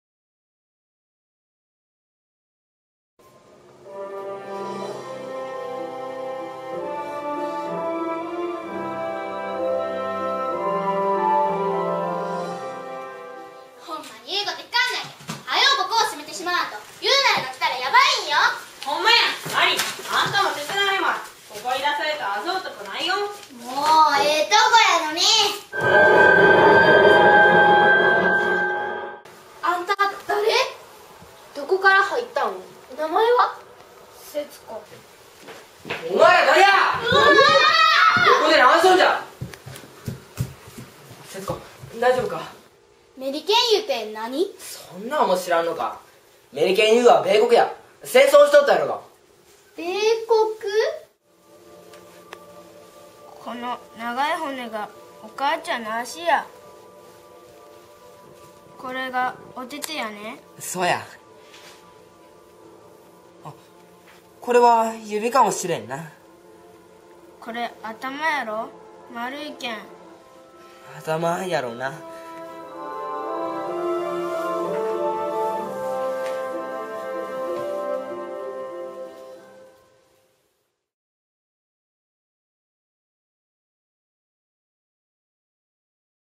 40億年の勇気.m4v
NPO法人アーツカウンシル高松主催の「子どものための演劇教室旗揚げ公演」「４０億年の勇気」が１０月３０日３１日両日、高松市通町３−５「あかつきホール」で開催されます。
屋島のふもとにある河川敷の古い洞穴を遊び場にしている子どもたちが、突然６５年前の高松にタイムスリップ。そこには、見たことも無い男の子と女の子二人。「高松は空襲で焼けてしもうた」兄妹の母の遺骨を握りしめ、戦争の愚かさを見守る子どもたち 。